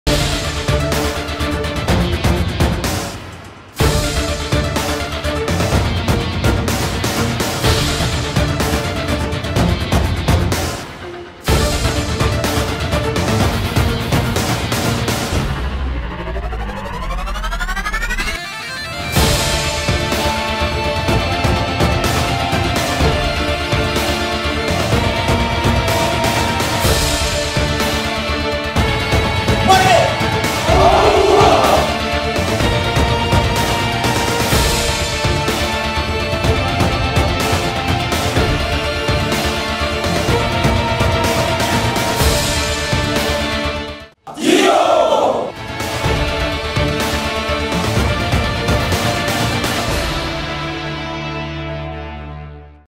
Wadan Pasmar 3 Pimpin Acara Penerimaan Purna Satgas Cendrawasih Jaya Tahap III Tahun 2024
Temukan Pasmar 3 Korps Marinir di